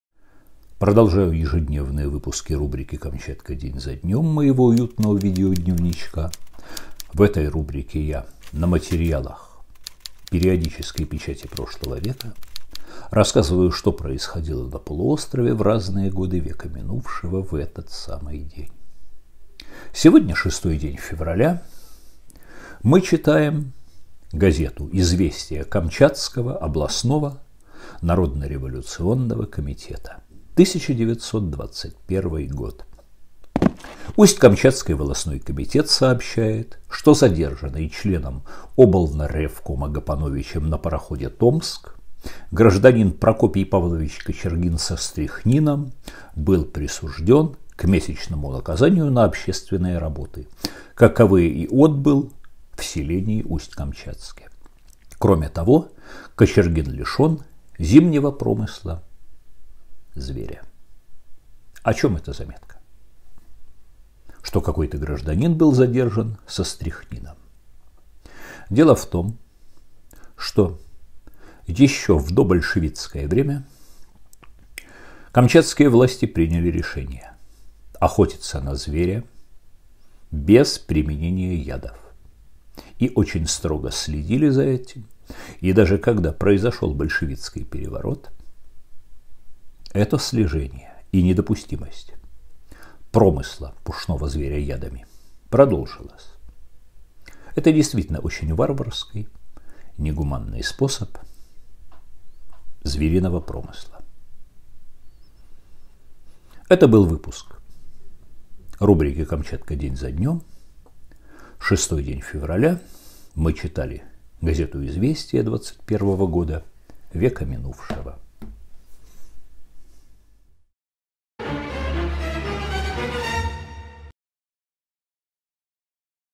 НА ПАРОХОДЕ ПЫТАЛИСЬ ПРОВЕЗТИ ЯД В УСТЬ-КАМЧАТСК ♥ НАРУШИТЕЛЬ НАКАЗАН ♥ ВЛОГ № 67, 6 ФЕВРАЛЯ 2021
На пароходе «Томск» был задержан гражданин, пытавшийся провезти яд для того, чтоб травить зверей.